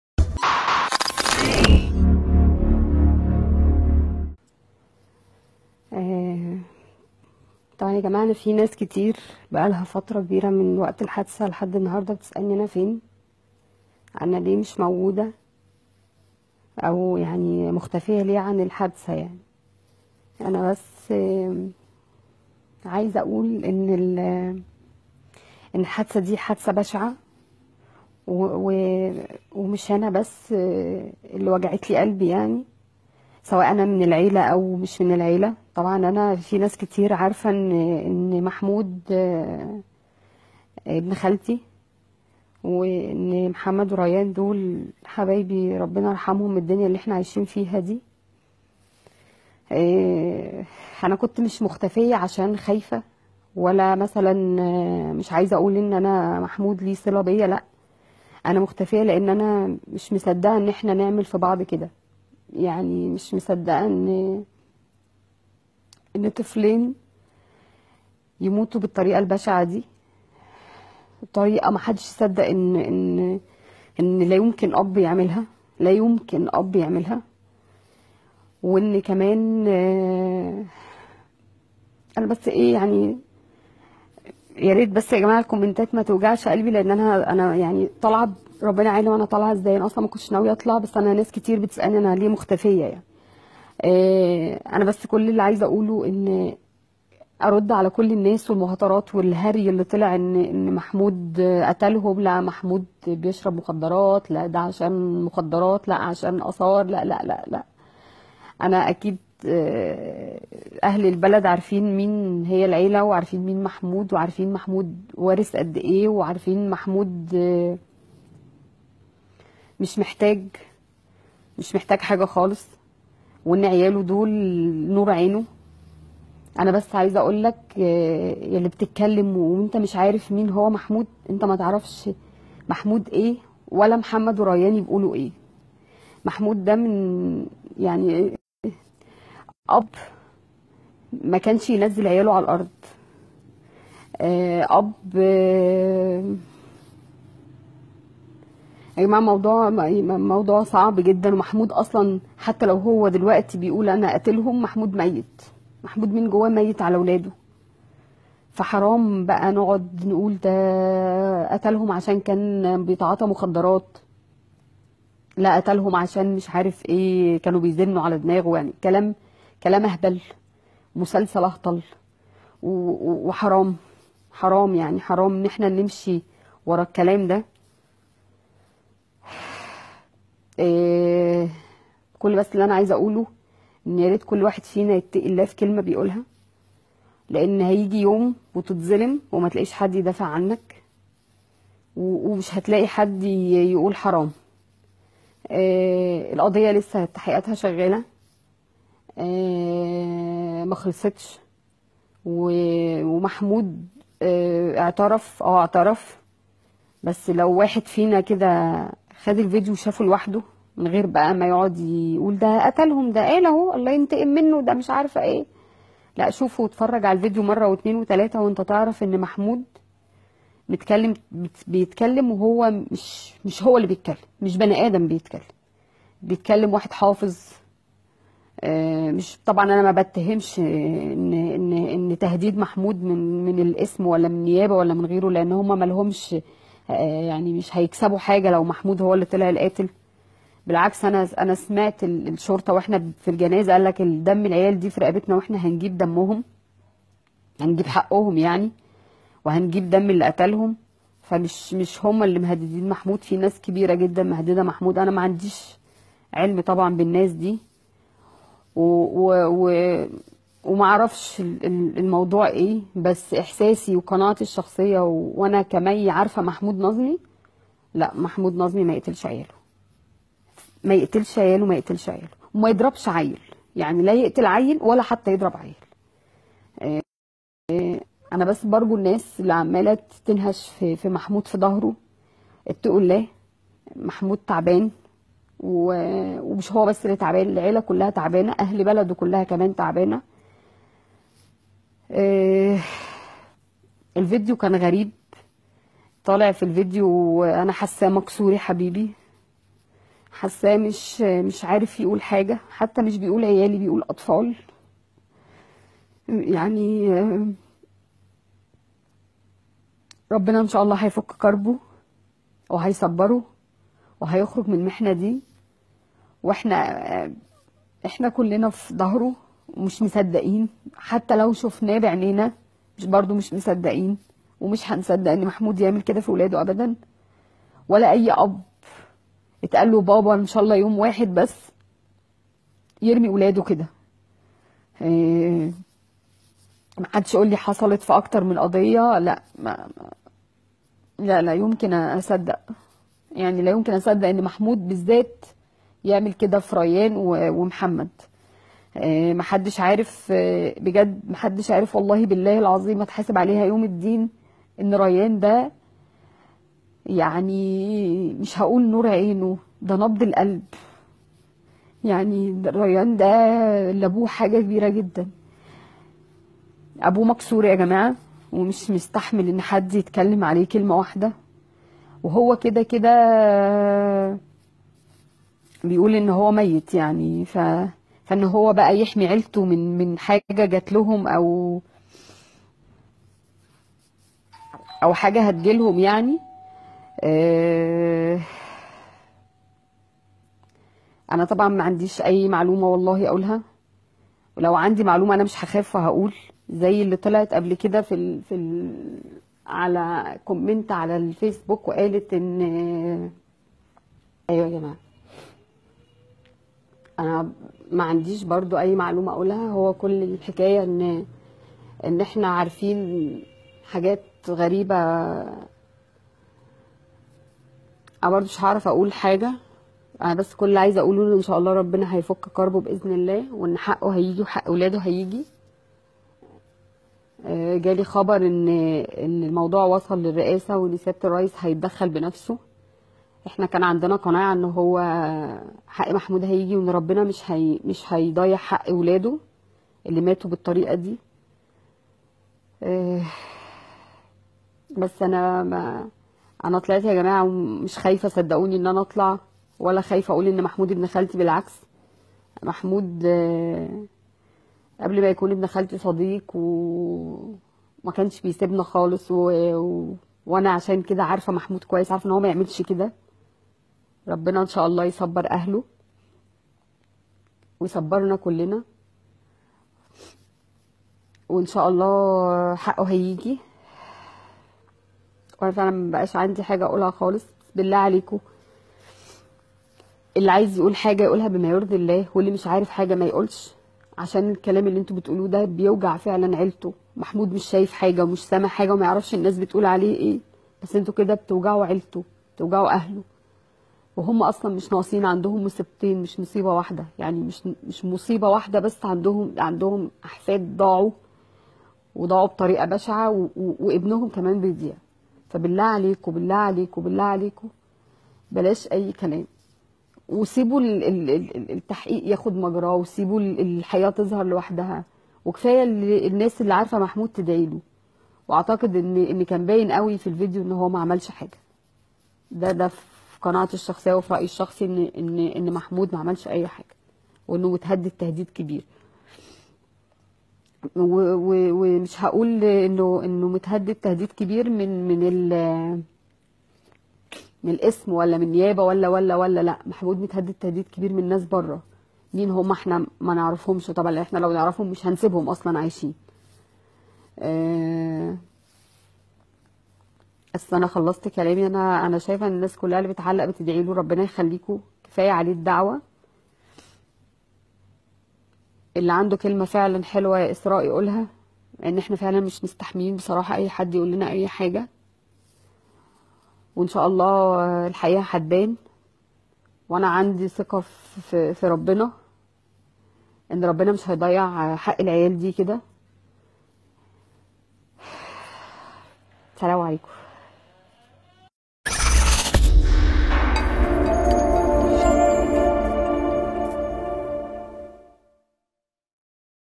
مفاجأة .. ظهور أم طفلي قرية ميت سلسيل المقتولين وتكشف اسرار وغموض الجريمة
للمزيد
فيس بوك :

تحت رعابة
Richard Elhaj Media